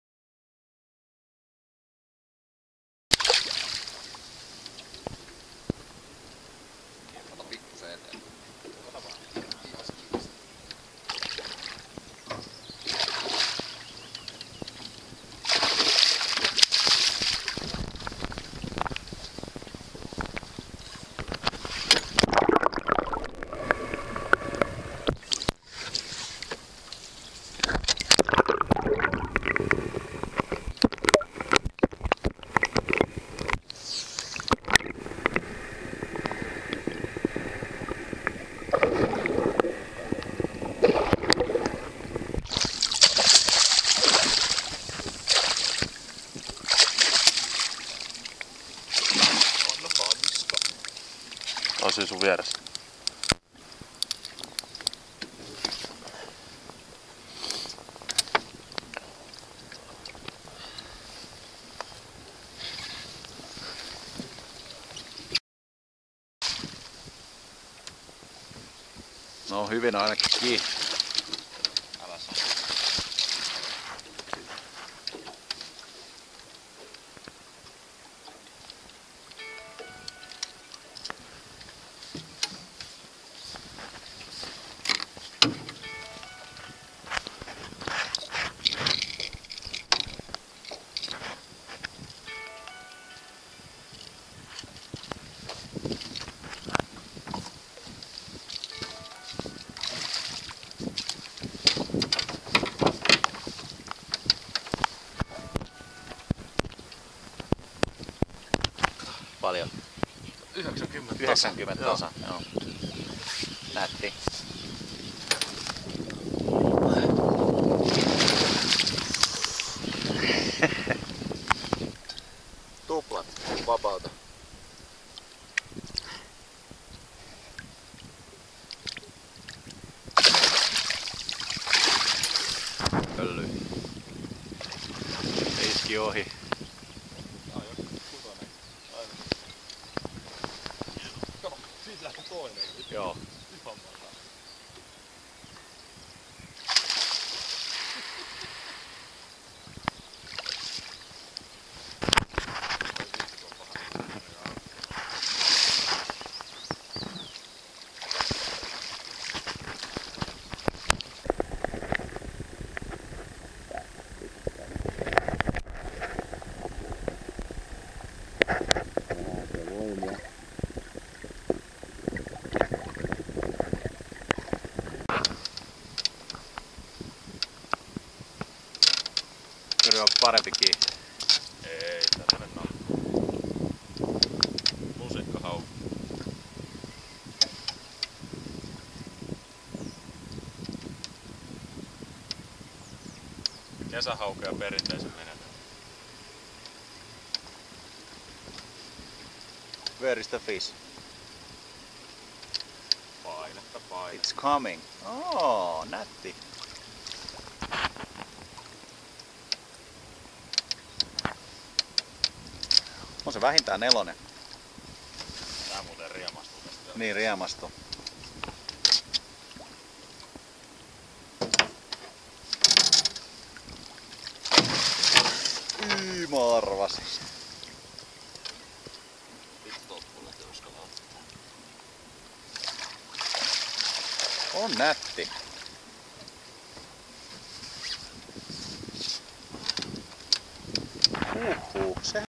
Haukea heittokalastamassa kevät 2011 Pike fishing in spring
Hauen kalastusta sisäsaaristossa heittäen jerkkiä,suspeja, sekä lusikaakin.